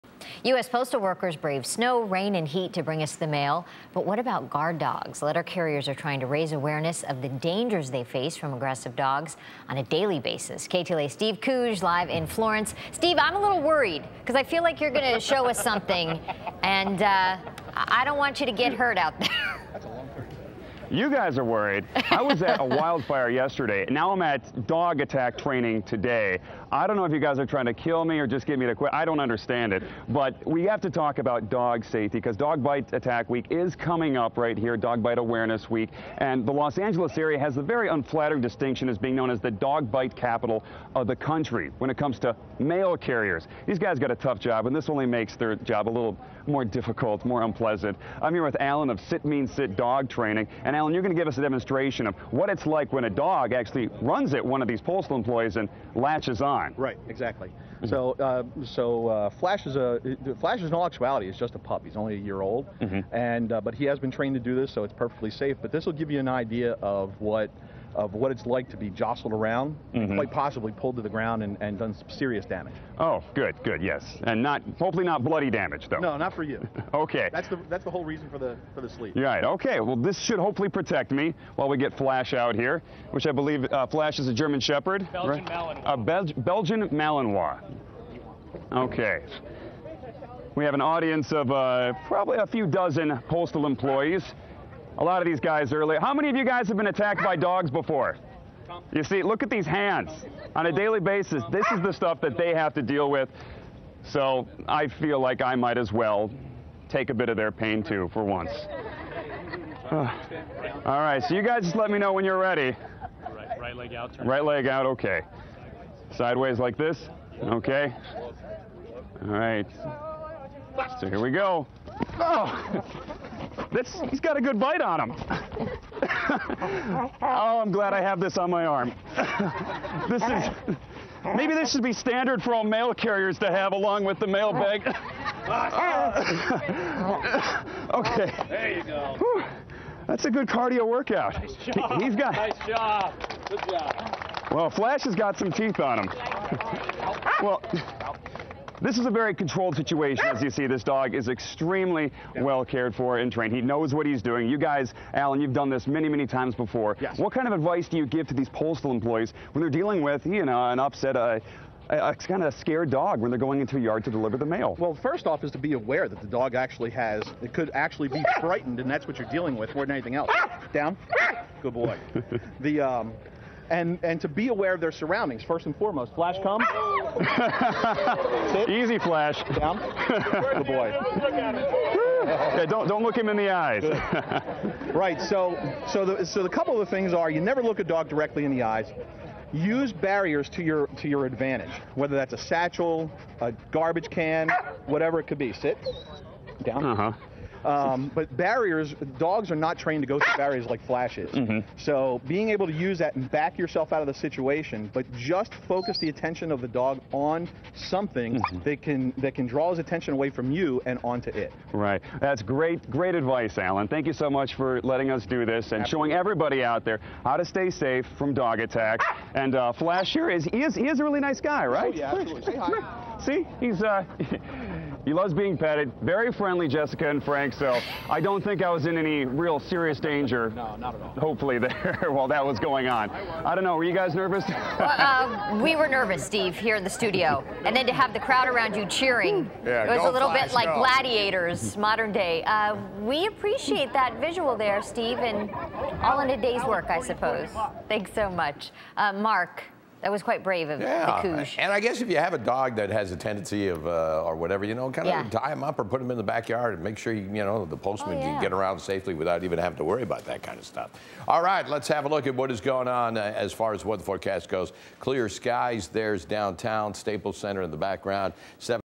Reporting 101: Getting Attacked by Dogs
My producers had someone sic a dog on me Live on TV. It was fun... except for the dog slobber on my pants afterwards.